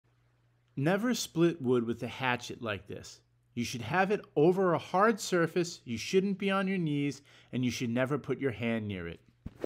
Never Split Wood With A Hatchet Like THIS
Ideally, you should split would with an axe or wood splitter, standing up, with the wood flat on hard surface like another log that is cut wide and shallow.